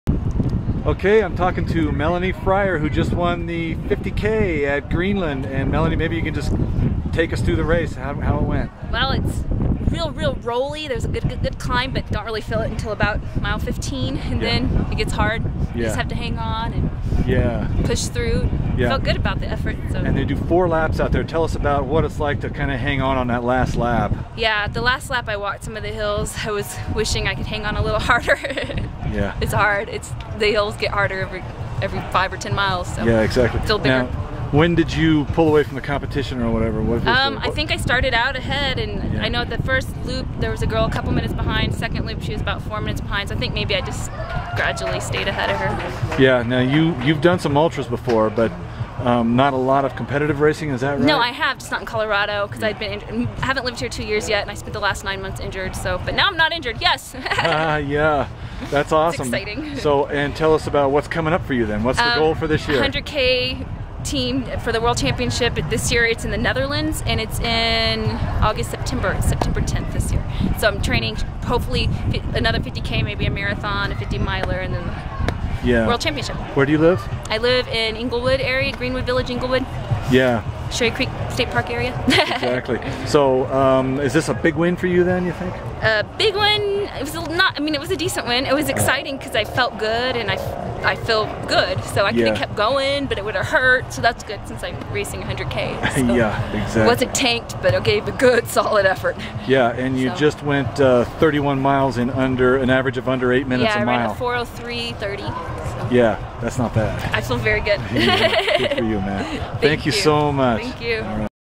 Melanie Fryar wins Greenland Trail 50K
Melanie Fryar of Greenwood Village captured the top spot in the Greenland Trail 50K on Saturday with a time of 4:03.30 - an average of about 7:51 per mile over 31 miles. Fryar led from the beginning and gradually built an advantage that helped on the last lap when she said she had to walk on some of the course's hilly sections. "The hills get harder every 5 or 10 miles," she said. Fryar was a member of the U.S. 100k team that competed in Spain last fall. She hopes to earn a spot on the team this year.